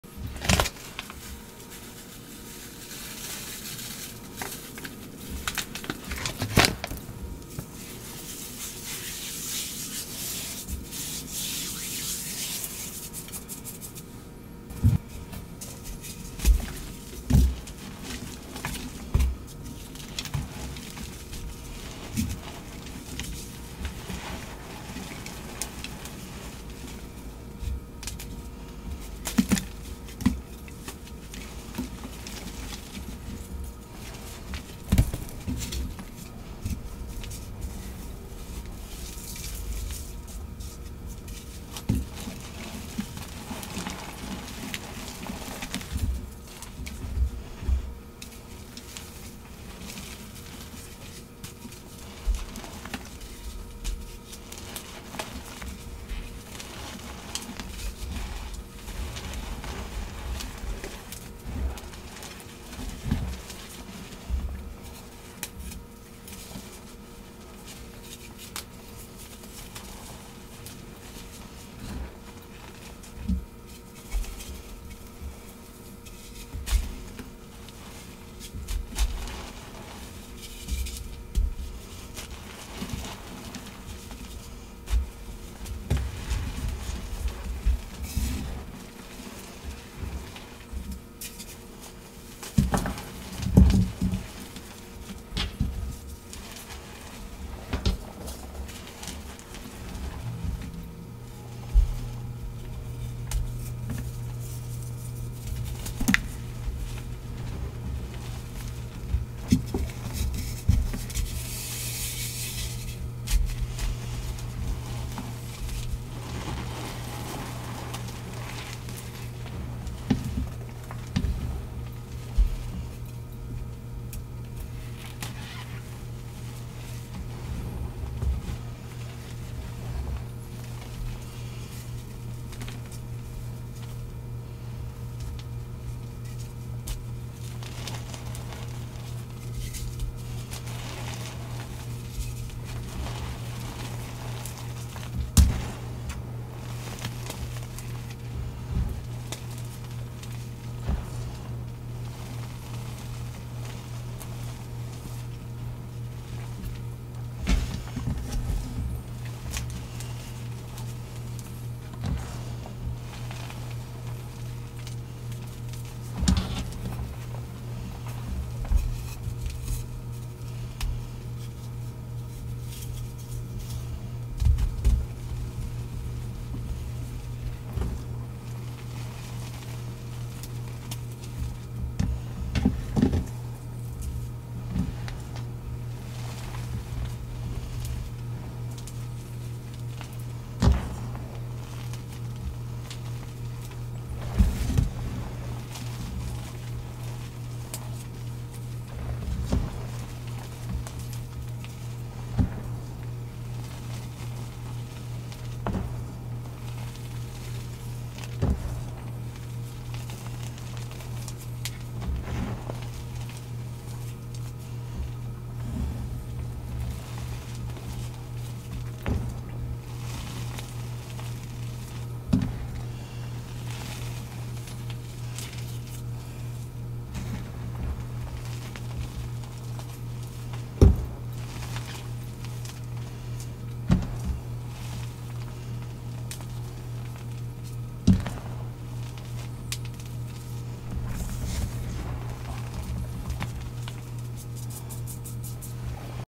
Super Soft Gym Chalk Crush ASMR
I loved this crush so much. The colors were pretty and the texture felt so good! Those bright yellow circles stole the show! 🟡

No edits or reposts on any platform.